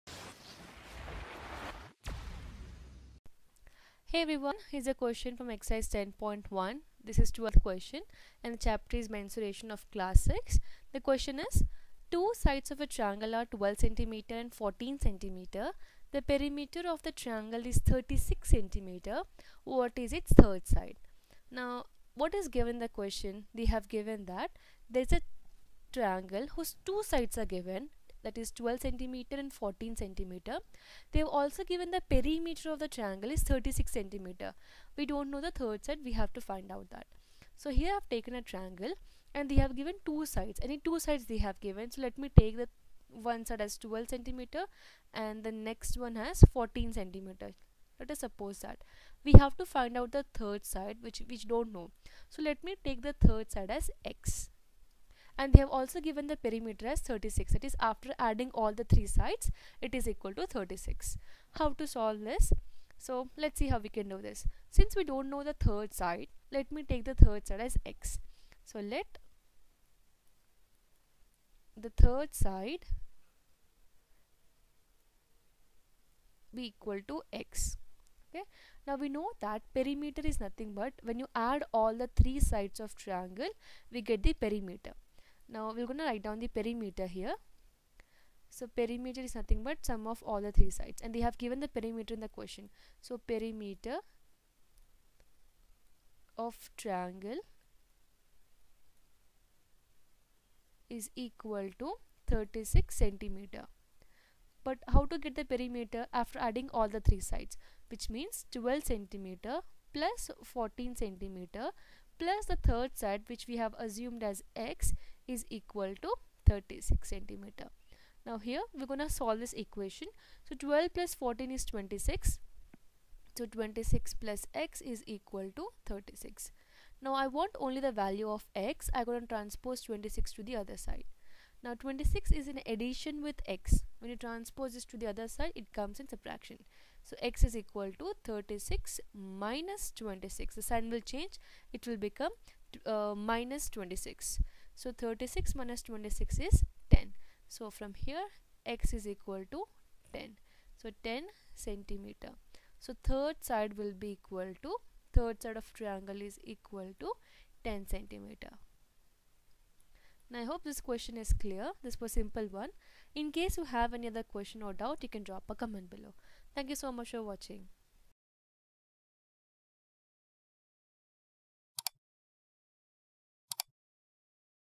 12q Ex10.1 Mensuration Class 6.Two sides of a triangle are 12 cm and 14 cm. The perimeter of the....
Two sides of a triangle are 12 cm and 14 cm. The perimeter of the triangle is 36 cm. What is its third side?